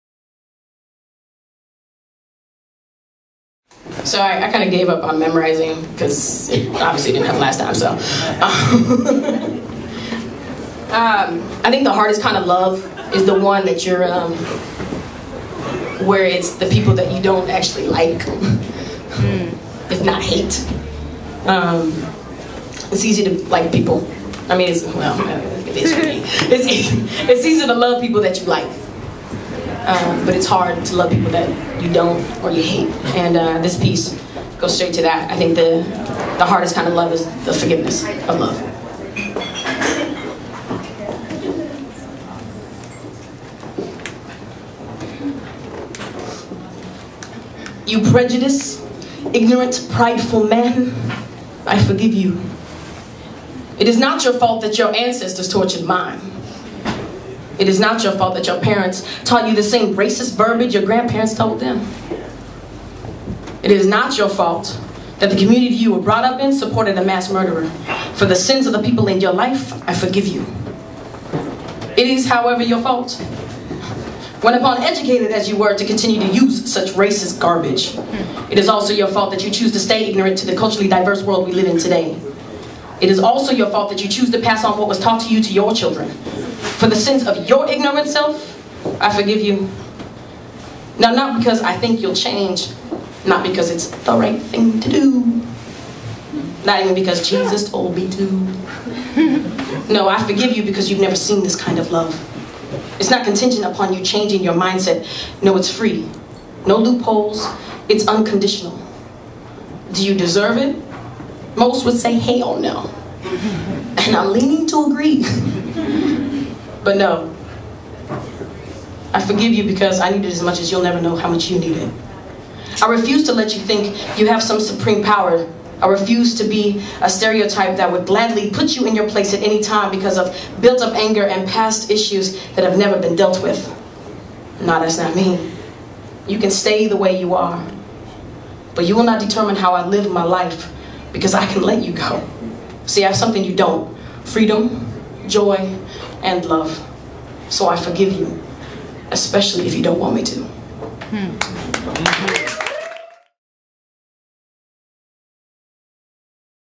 Courtney Nikki (2) @ The Cornerstone Open Mic
April 2011 Featured Poet: Courtney Nikki
"I Forgive You"

Every 1st Saturday of the month @ Faire Gallery & Cafe. Spoken Word, Freestyle Cyphers, Artist Showcases, a DJ, and a Full Live Band.